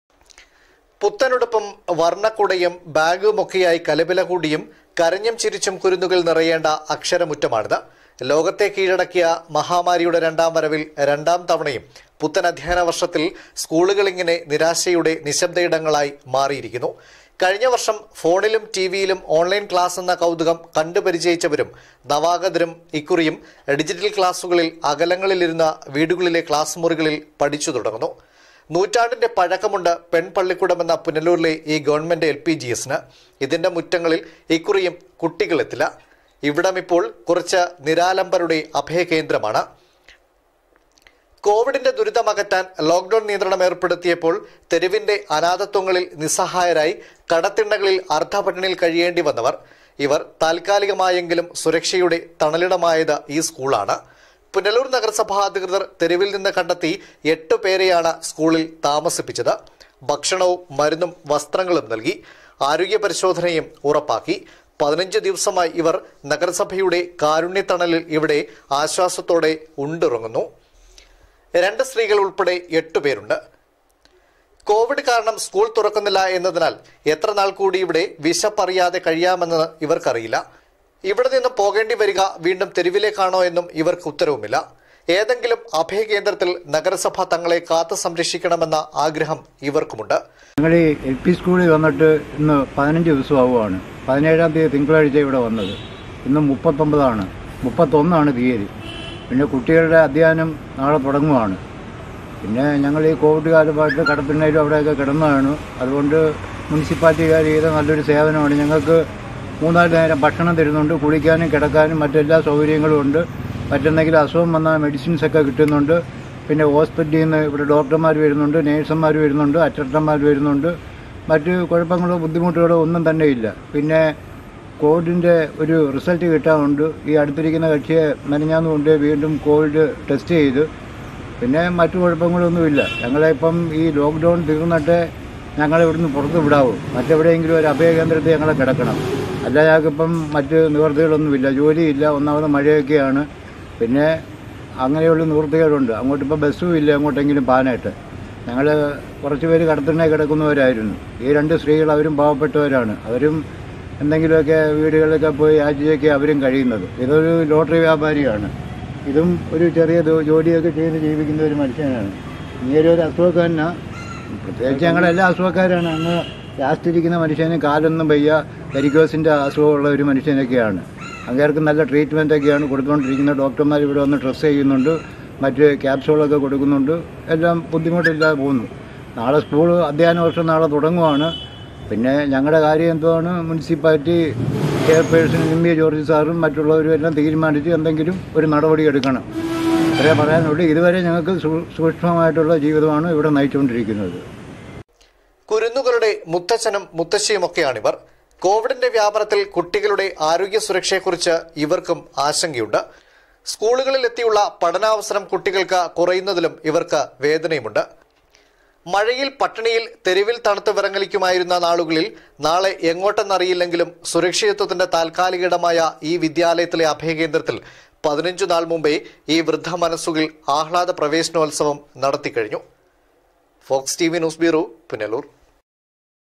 FOX TV
വർണ്ണ കുടയും ബാഗും ഒക്കെയായി കലപില കൂട്ടിയും കരഞ്ഞും ചിരിച്ചും ഒരിക്കൽ നിറയേണ്ട അക്ഷരമുറ്റമാണിത് ലോകത്തെ കീഴടക്കിയ മഹാമാരിയുടെ രണ്ടാം വരവിൽ രണ്ടാംതവണയും പുത്തൻ അധ്യായന വർഷത്തിൽ  സ്കൂളുകൾ ഇങ്ങനെ നിരാശയുടെ നക്ഷത്രങ്ങൾ ആയി മാറിയിരിക്കുന്നു